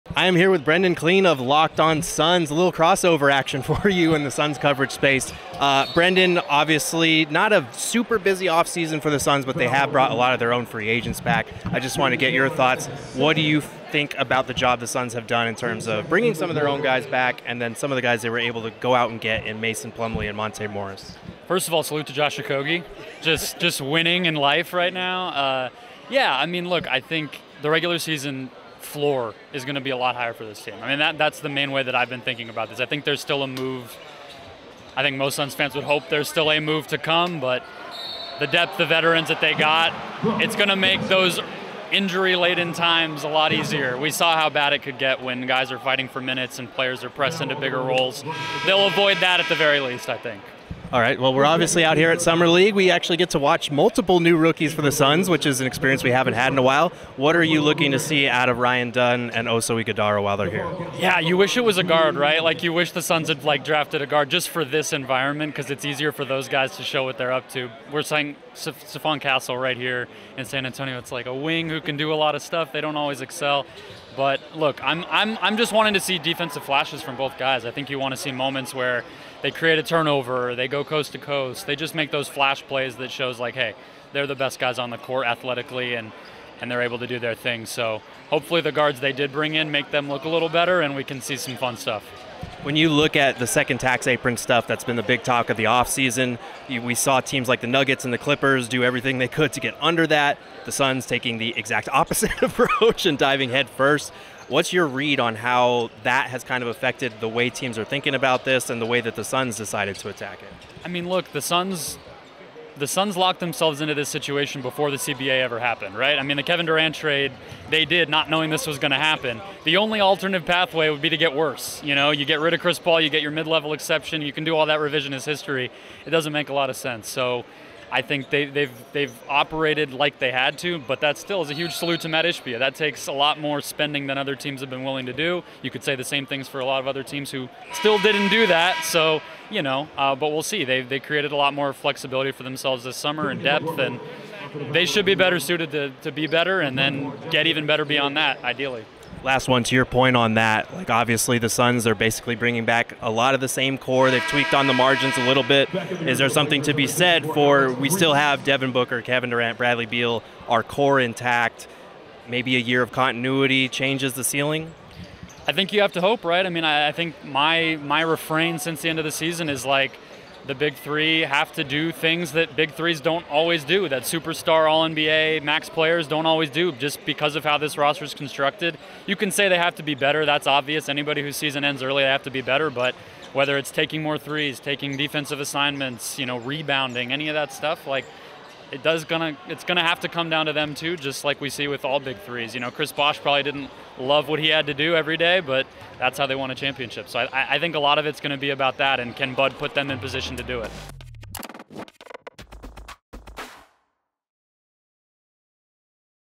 Locked On Suns NBA reporter, Brendon Kleen On Phoenix Suns Offseason & Their Future
Locked On Suns NBA reporter, Brendon Kleen spoke with PHNX Suns' Gerald Bourguet at 2024 NBA Summer League about the Phoenix Suns offseason. The Suns Rookies, Ryan Dunn and Oso Ighodaro.

PHNX Locker:
PHNXLocker.com